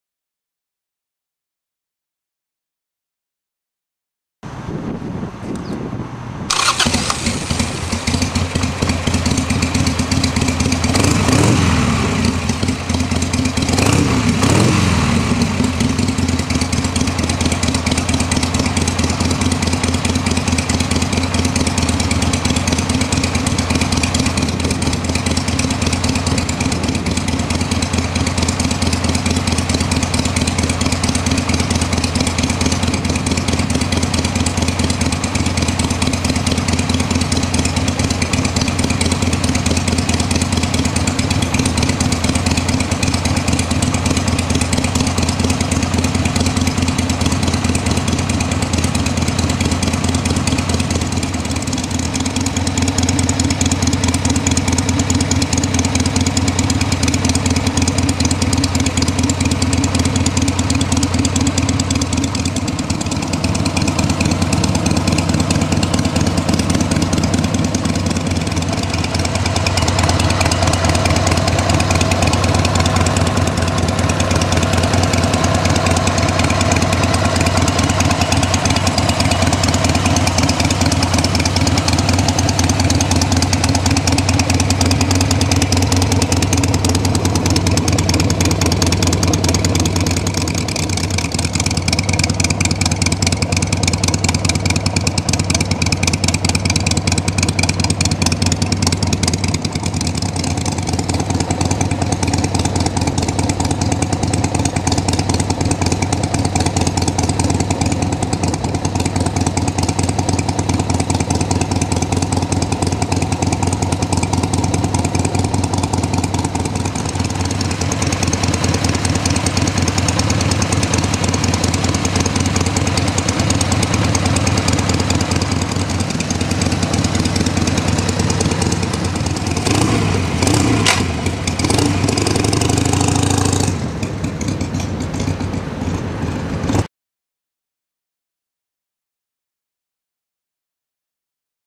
2001 XLH-883 Sportster® 883 38486 ChopperExchange.com
ChopperExchange.com/38486
See this bike and 7000+ others at ChopperExchange.com

2001 XL883
Stock
Luxury Blue, 4000 miles
$4,995